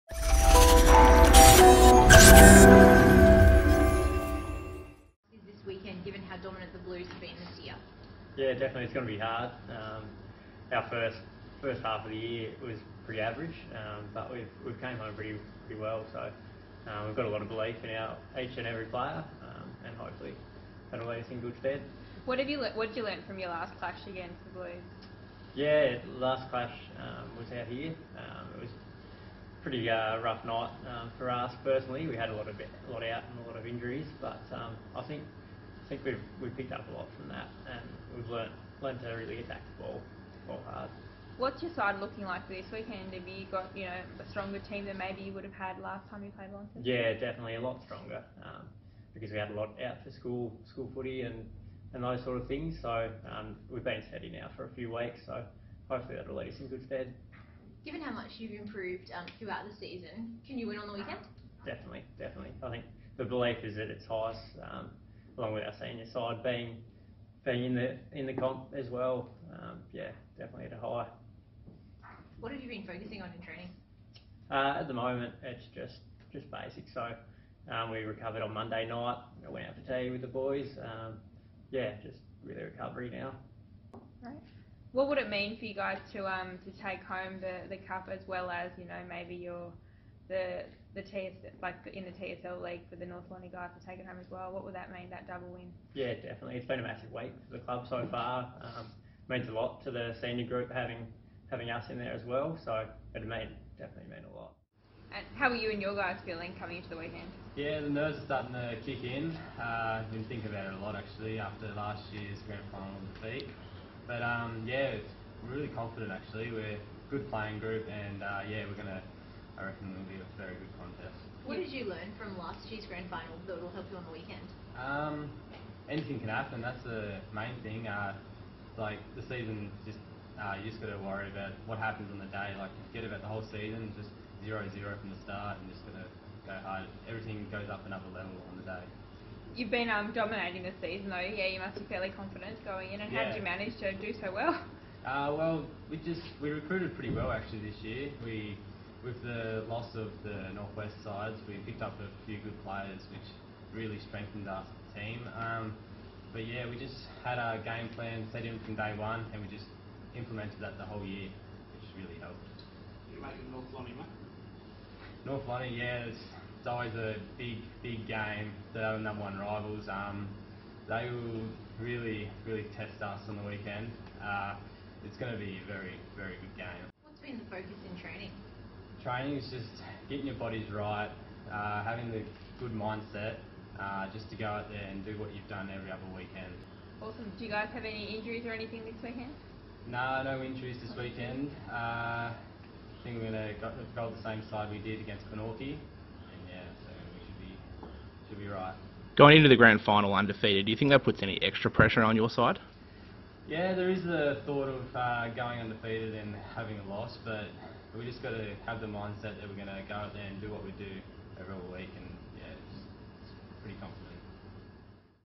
Mercury Cup Grand Final Media
Launceston's Grant Holt and North Launceston skipper Nathan Jackson face the media ahead of Saturday's MercuryCup Grand Final at UTAS Stadium.